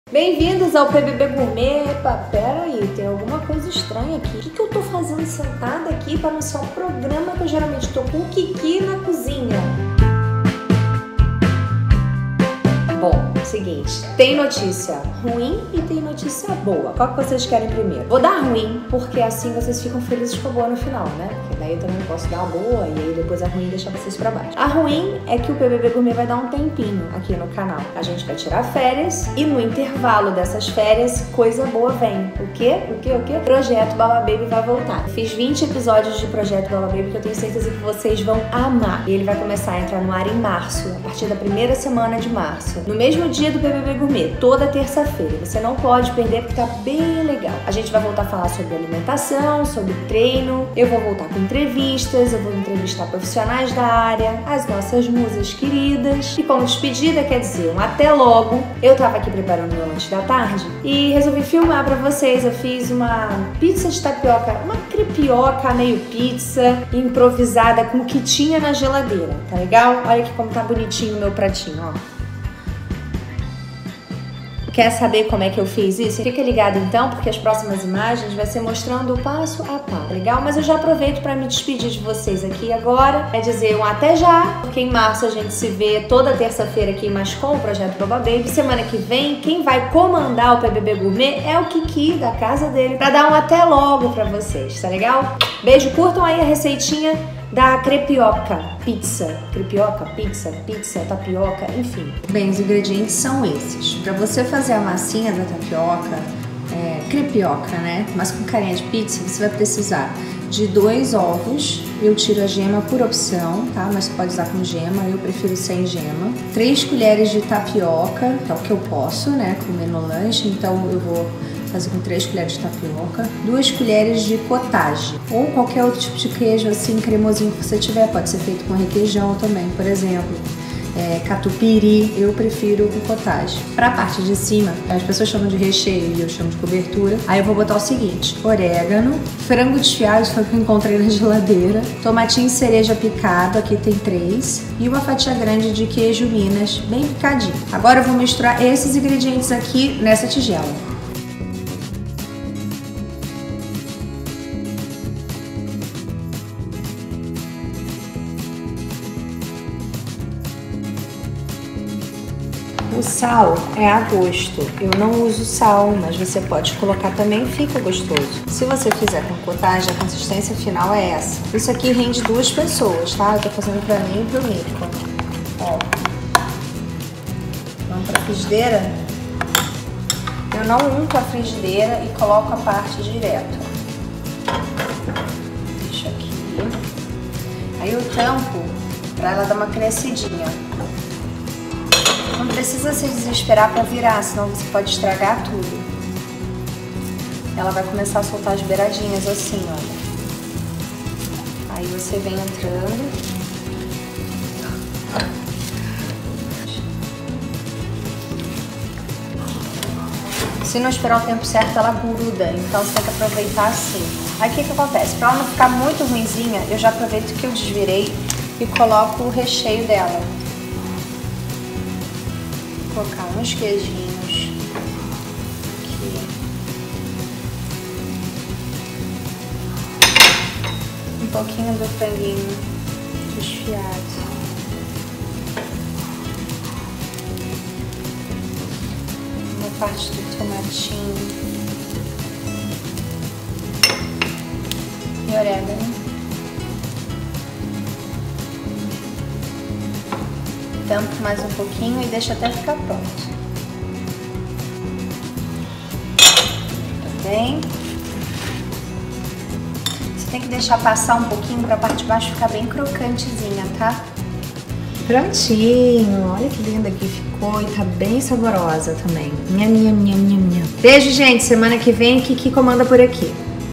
PBB Gourmet #75 - PIZZA, CREPIOCA OU TAPIOCA? (+NOVIDADE)
Uma opção rápida, prática e gostosa pro seu lanche.

INGREDIENTES
2 ovos (retirar a gema é opcional)
3 col. sopa de tapioca
2 col. sopa de Cottage
Orégano (a gosto)
Tomate cereja (a gosto)
Queijo (a gosto)
Frango desfiado (a gosto)

Fiquem ligados porque todas as terças teremos novas receitas fit pra vocês!